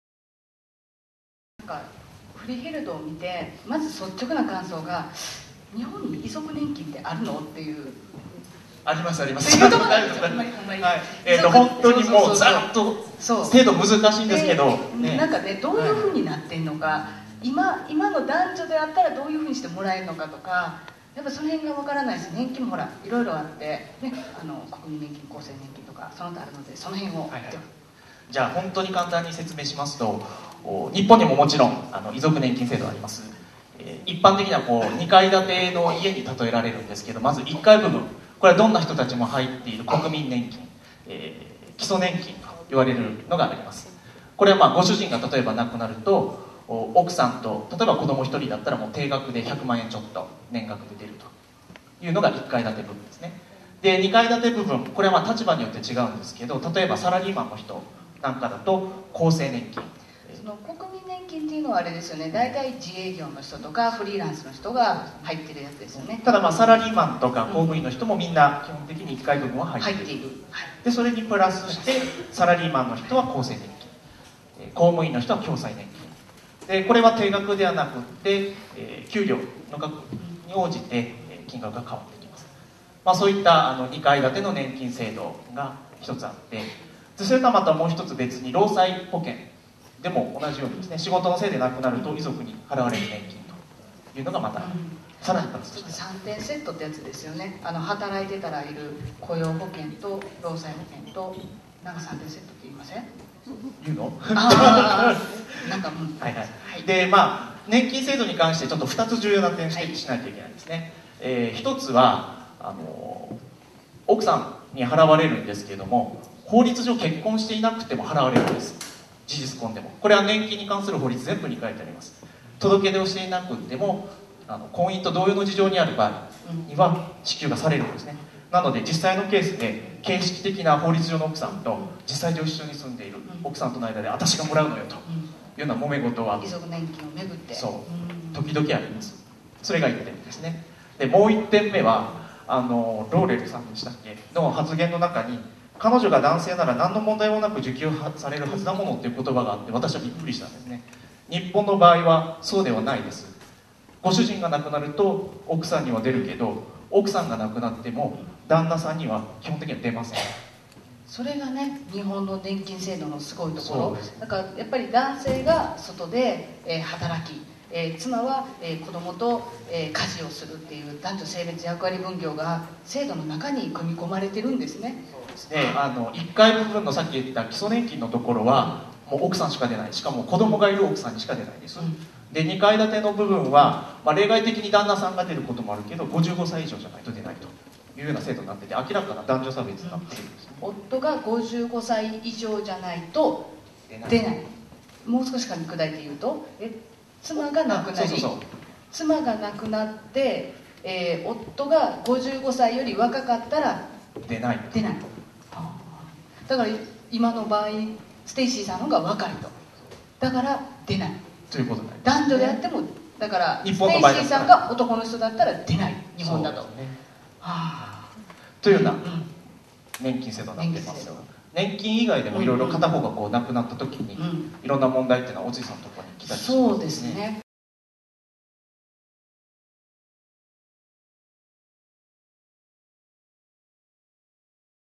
アカデミー賞受賞ドキュメンタリー「フリーヘルド/Freeheld」上映後TALK02～尾辻かな子さん& 山下敏雅さん●日本の年金制度の基本理念の問題点
2008年7月20日(日)。青山スパイラルホールで開催された東京国際レズビアン＆ゲイ映画祭。アメリカのドキュメンタリー映画「フリーヘルド/Freeheld」上映後に行われたTALKです。
【TALK出演】
尾辻かな子さん（前大阪府議会議員）
山下敏雅さん（弁護士/東京パブリック法律事務所）
 ●こちらの記事と連動しています。
(Ｃ)akaboshi
「フツーに生きてるＧＡＹの日常」
●第17回東京国際レズビアン＆ゲイ映画祭
The 17th Tokyo International Lesbian & Gay Film Festival

『フリーヘルド』2007 / USA / 38min / Language: English

本年度アカデミー賞受賞！　魂ゆさぶる感動作、緊急上映！

25年間ニュージャージー州で警官として勤務したローレルは、49歳でガンにより余命半年を宣告される。パートナーのステイシーが遺族年金を受け取れるよう地元オーシャン郡に申請を出すが、同性であるがため却下されてしまう。残された日々をかけがえのない絆を守るために戦いぬく決意をしたローレルの勇気は、人々を動かし、社会を動かした。そして今、観るものすべての記憶に深く刻まれるだろう...。アカデミー賞短編ドキュメンタリー映画賞、サンダンス映画祭特別審査員賞受賞作品。

Freeheld / A Lez In Wonderland (ASIAN PREMIER)
Freeheld - 2007 / USA / 38min / Language: English

The fight for queer equality has not been won

Detective Lieutenant Laurel Hester devoted 25 years of her life to law enforcement in Ocean County, New Jersey. Then when she was diagnosed with terminal cancer she applied to the local elected officials, the Ocean County Freeholders, to have her partner, Stacie Andree, receive her pension. Although an automatic right for heterosexual partners, she was rejected. Freeheld chronicles the community's fight for justice and Laurel's declining health. Freeheld does more -- it introduces a truly devoted couple at the most tragic time of their lives.